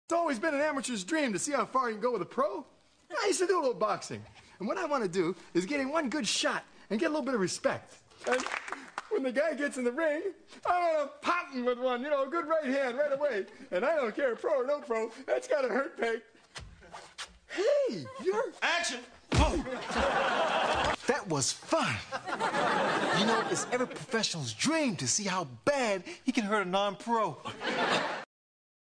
UFC - CM Punk vs. Mickey Gall
Al Bundy as CM Punk, Sugar Ray as Mickey Gall.